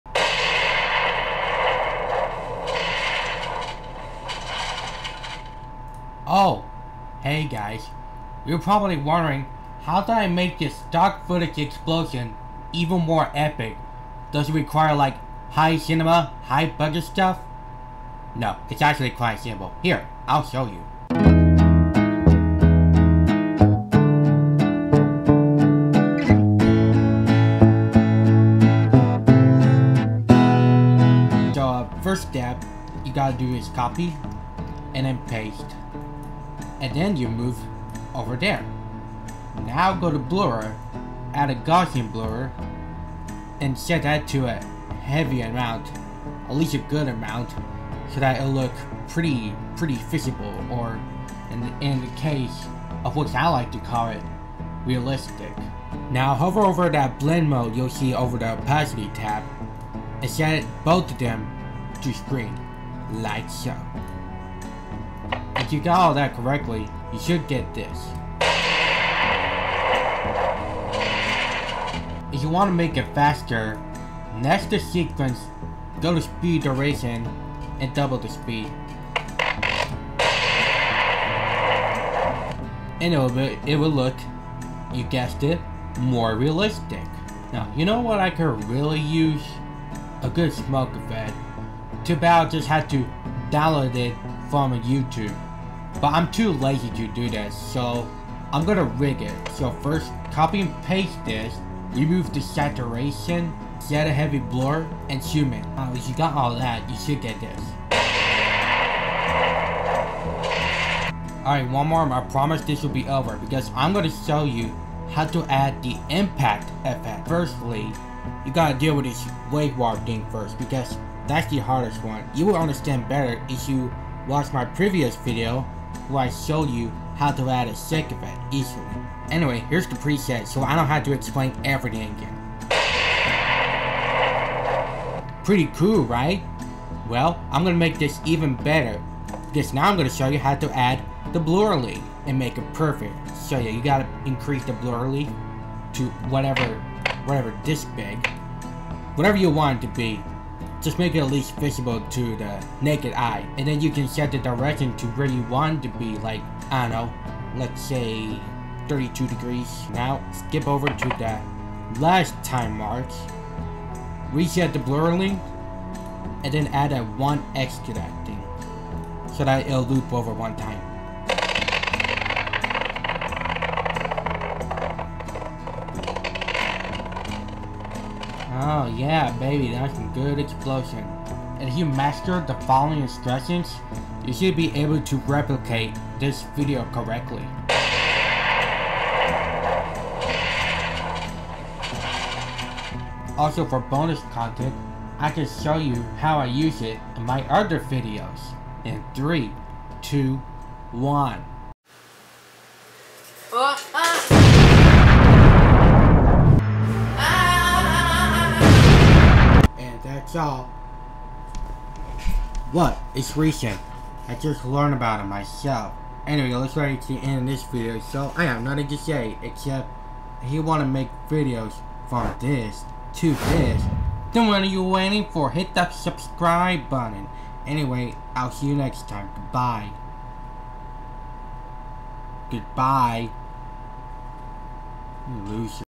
MrBeast's stock Explosion, but more epic - Adobe Premiere Tutorial
8/2/24 - woe is me.

This is the second episode from my premiere tutorial series, in honor of @MrBeast becoming the most popular youtuber in the world.

shoutout @animatedxtream9292 because that's what you see in every mrbeast video

0:00 preview + intro
0:31 as simple as possible
1:27 bonus smoke effect
1:51 bonus impact effect
3:03 all three at once
3:21 times I used it in my videos
3:30 the end